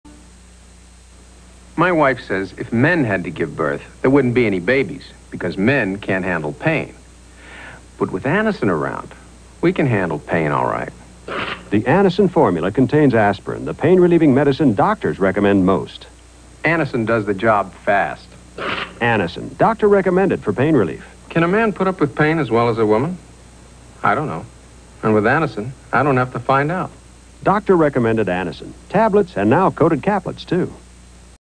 1986 Anacin "If men had to give birth" TV Commercial
Not quite sure if this one still works today.

This commercial aired on October 3rd 1986. It was shown on during the broadcast of "Dallas" on Channel 3 WRBL.